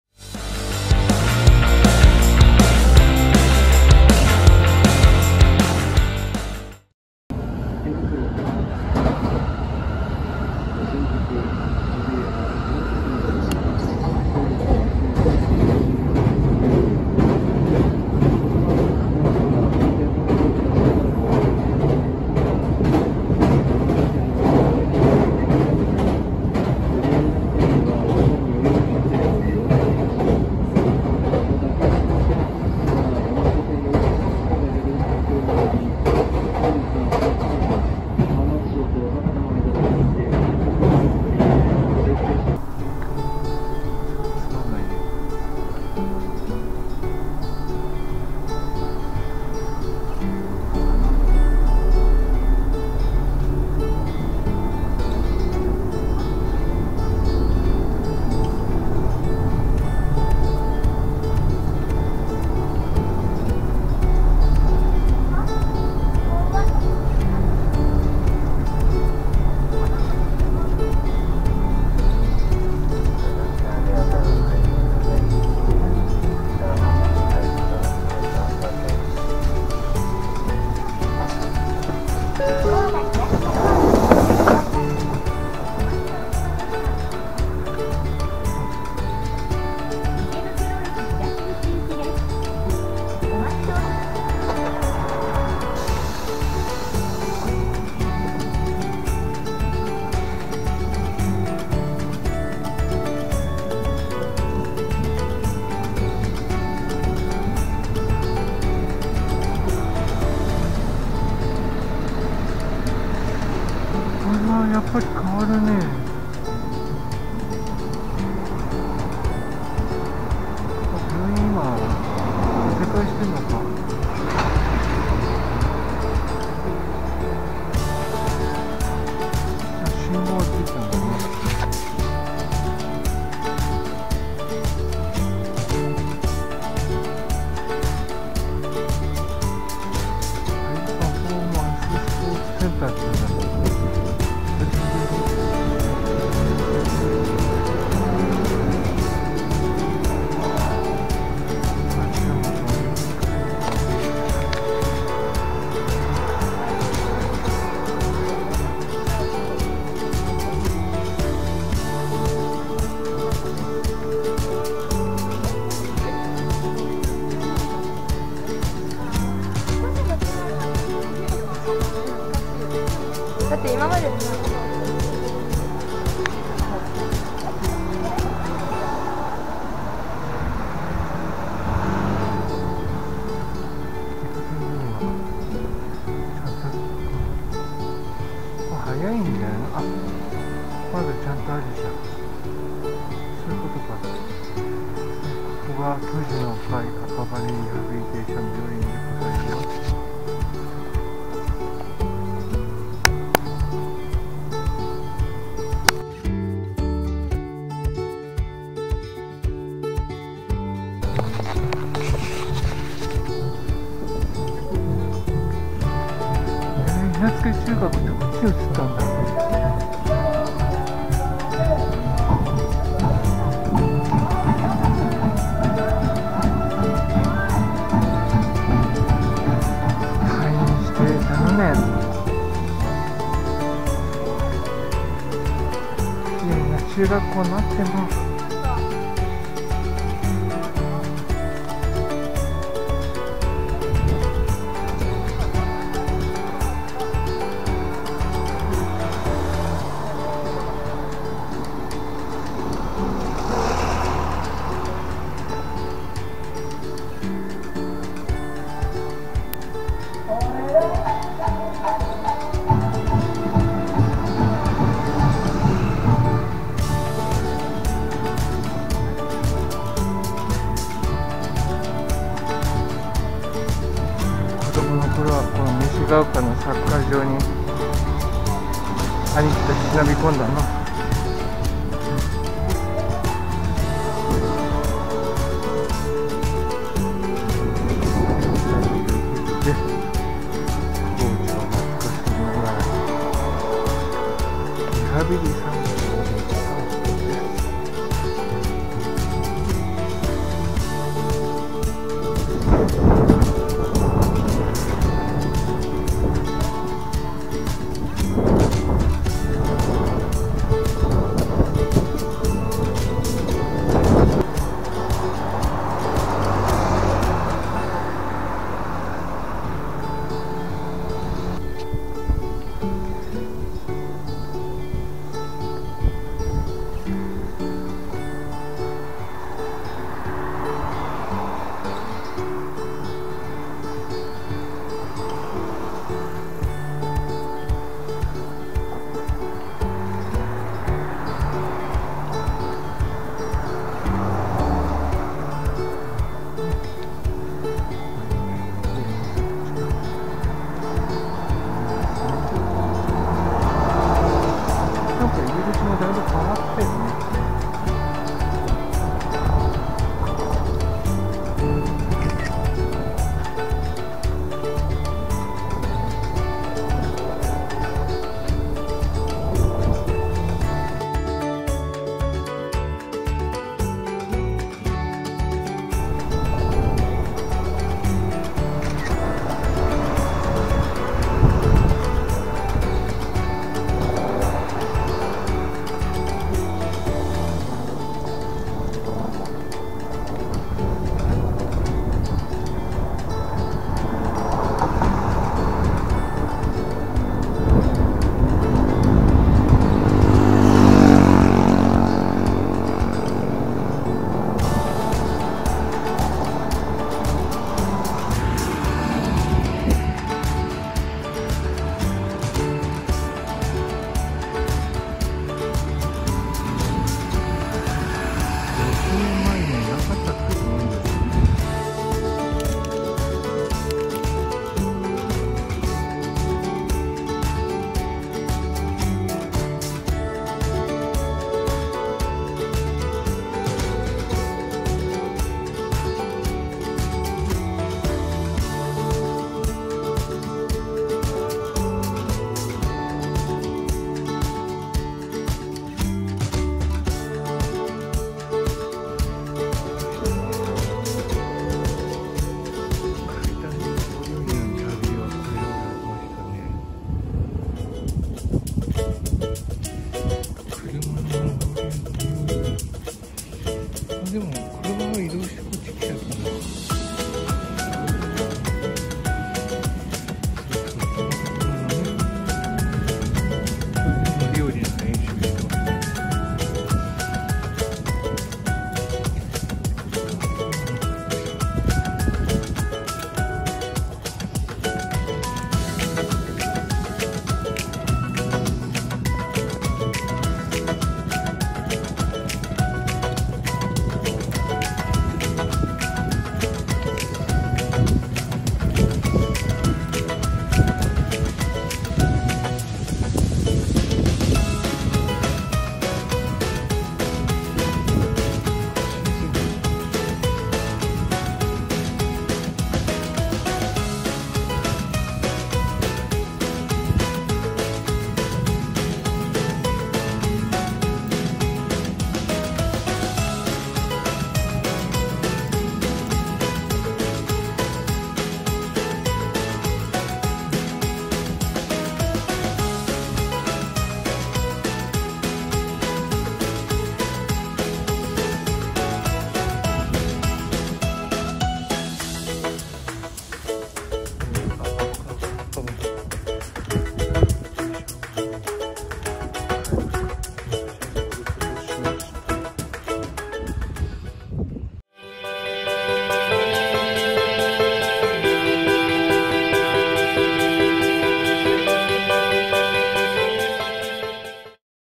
リハビリ散歩〜回復期のリハビリ病院を訪ねて〜
回復期のリハビリ病院でやっていたリハビリの一つの味の素ナショナルトレーニングセンターの外周を歩行したいと思って訪ねました。6年ぶりに降りたその場所は当時とはかなり変わっていました。記憶をたどりながらのリハビリ散歩です。